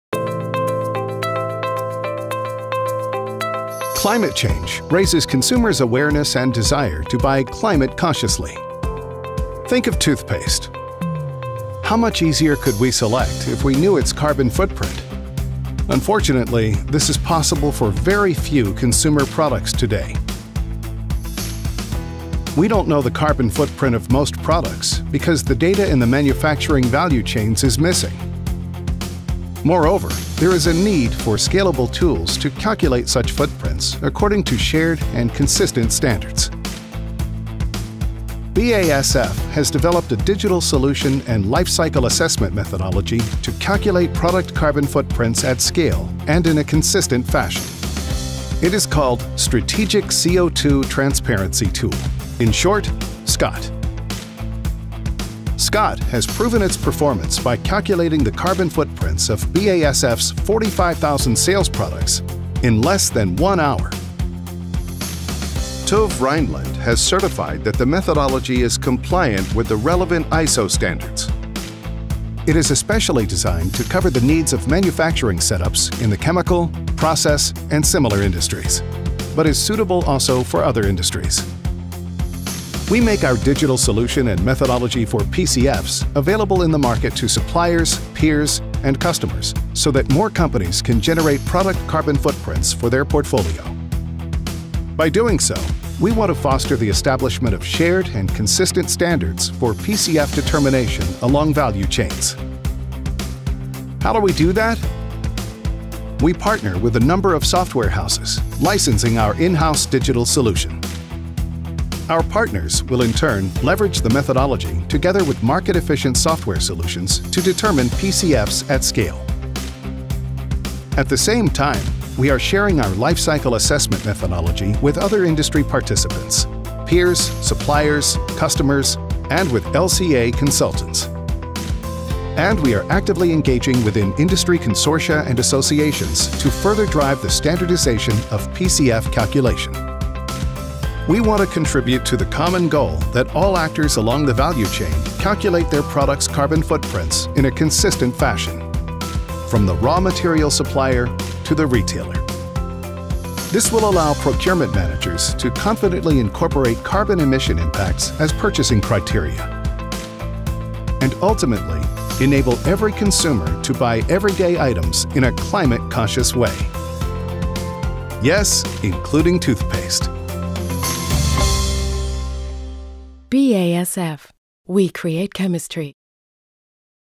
Creating carbon transparency on product level
BASF’s methodology and proprietary digital solution for product carbon footprint calculation has proven its usability beyond our company’s scope. We have shared it with partnering software houses via licensing agreements. Based on BASF’s methodology, our partners develop software for implementation at third parties.
Steering the reduction of greenhouse gas emissions towards net zero requires the identification of reduction potential in value chains. Gaining transparency with carbon footprint on product level is an essential precondition to work towards that goal. BASF has therefore developed a proprietary digital solution to efficiently calculate product-related cradle-to-gate carbon footprints (PCF) leveraging primary data. BASF’s solution enables the company not only to provide accurate and granular PCFs for its sales portfolio, but also creates unparalleled transparency in what is driving greenhouse carbon emissions in its manufacturing footprint and raw material purchasing basket.